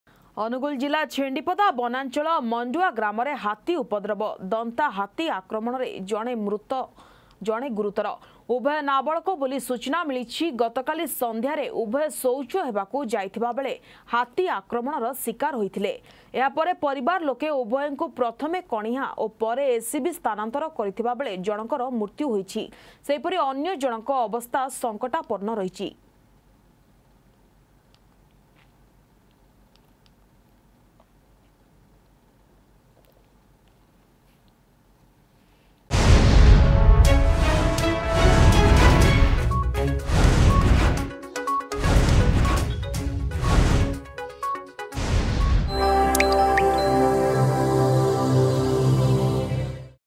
ଅନୁଗୋଳ ଜିଲ୍ଲା ଛେଣ୍ତିପଦା ବନାଞ୍ଚଳ ମଣ୍ତୁଆ ଗ୍ରାମରେ ହାତୀ ଉପଦ୍ରବ
ଅନୁଗୋଳ ଜିଲ୍ଲା ଛେଣ୍ତିପଦା ବନାଞ୍ଚଳ ମଣ୍ତୁଆ ଗ୍ରାମରେ ହାତୀ ଉପଦ୍ରବ । ଦନ୍ତାହାତୀ ଆକ୍ରମଣ ଜଣେ ମୃତ ଜଣେ ଗୁରୁତର । ଉଭୟ ନାବାଳକ ବୋଲି ସୂଚନା ମିଳିଛି.
ଅଧିକ ଖବର ପାଇଁ ଡ଼ାଉନଲୋଡ କରନ୍ତୁ “Argus News” ଆପ୍